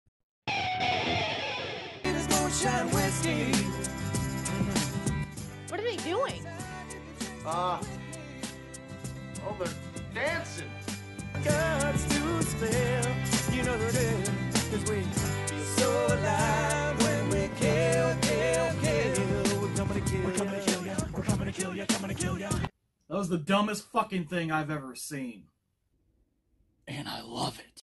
everybody DIES! (short version) - Episode #003: Dead & Breakfast
Welcome to a shortened sample version of my horror review show called everybody DIES!